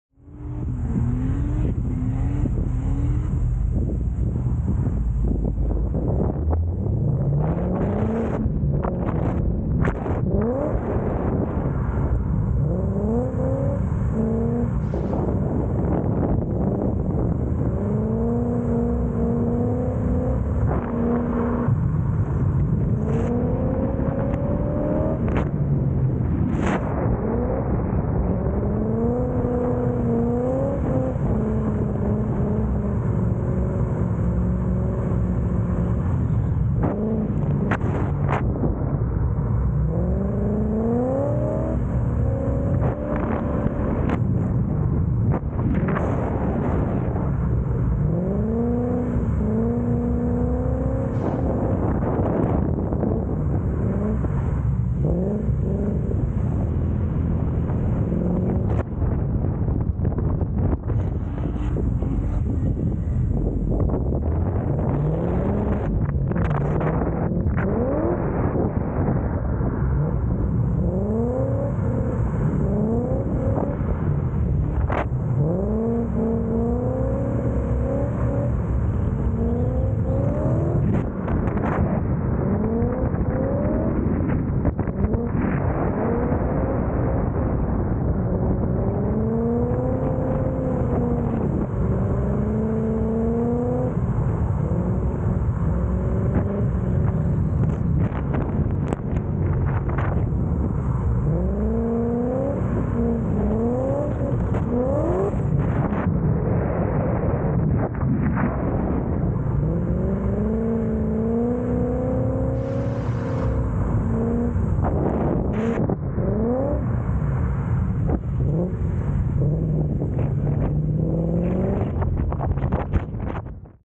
20231029 Drifting
A bit of drifting on the low-speed course in Lelystad. It has been quite a while since I've been here.